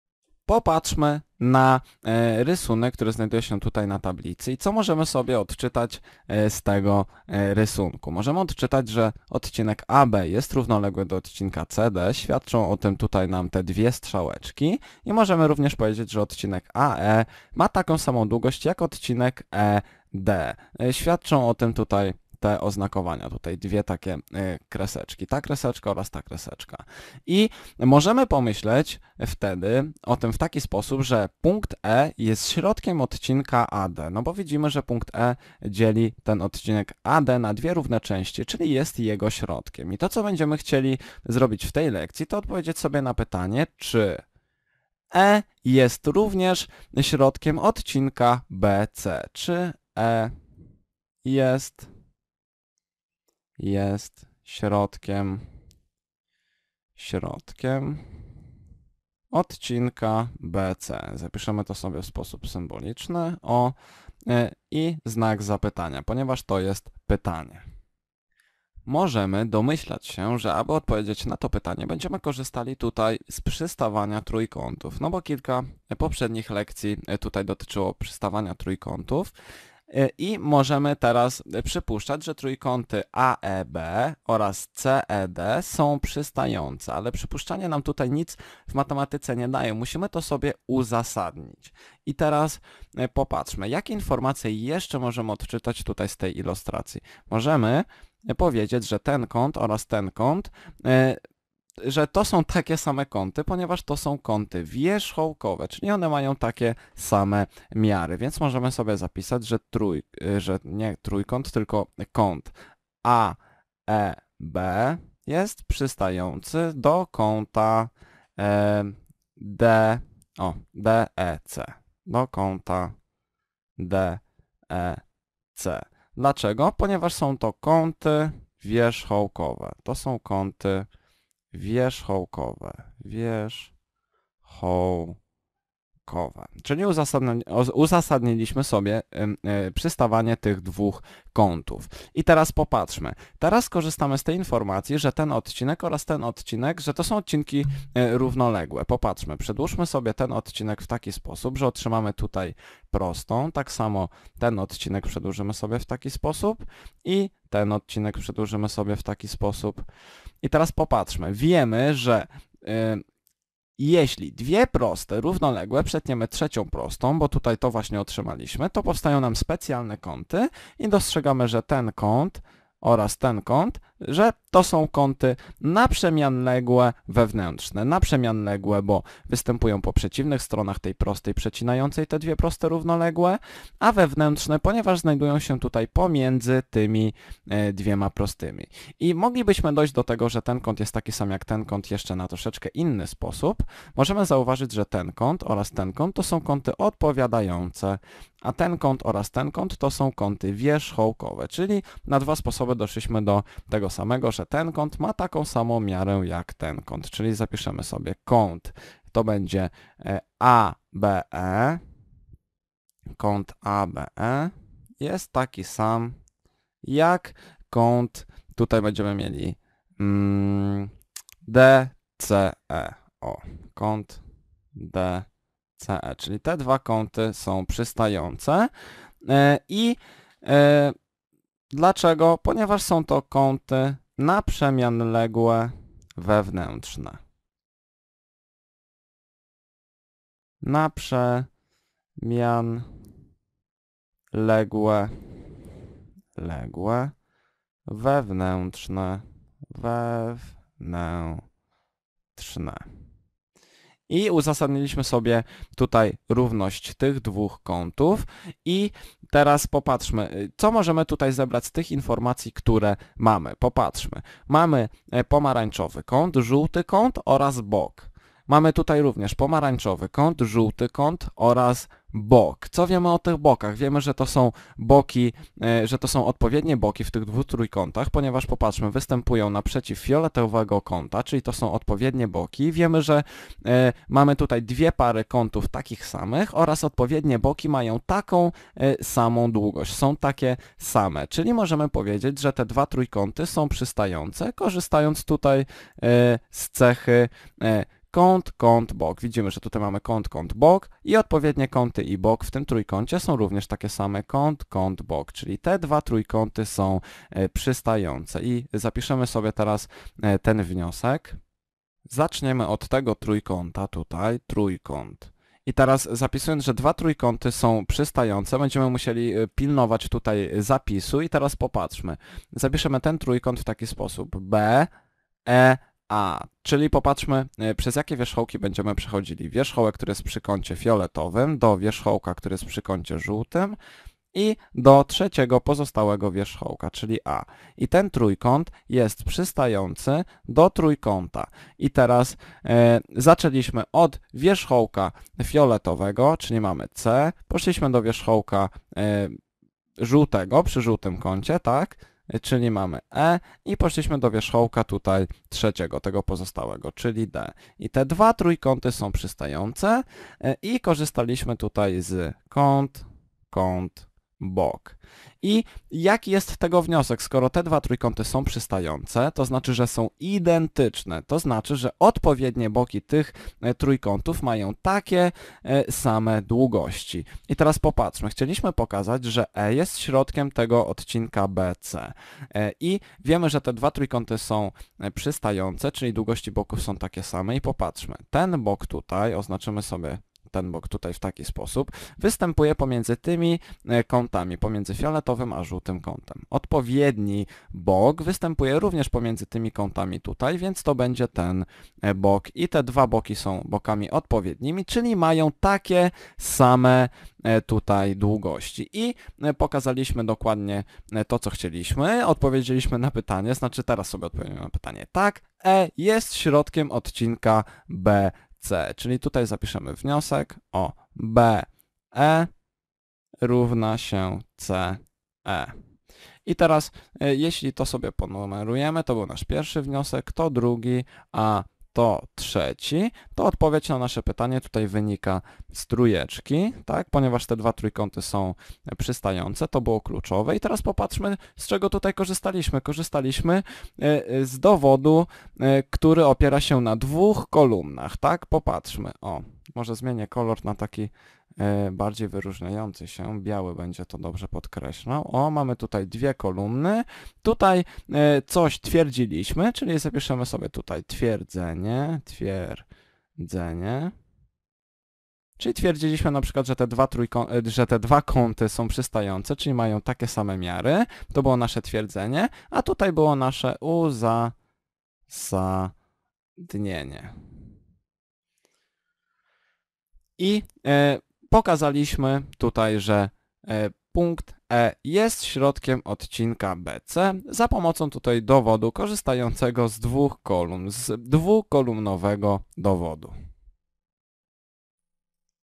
Wykorzystanie przystawania trójkątów w dowodzie twierdzenia
na zlecenie Centrum Fizyki Teoretycznej PAN. Narracja: Krzysztof Chojecki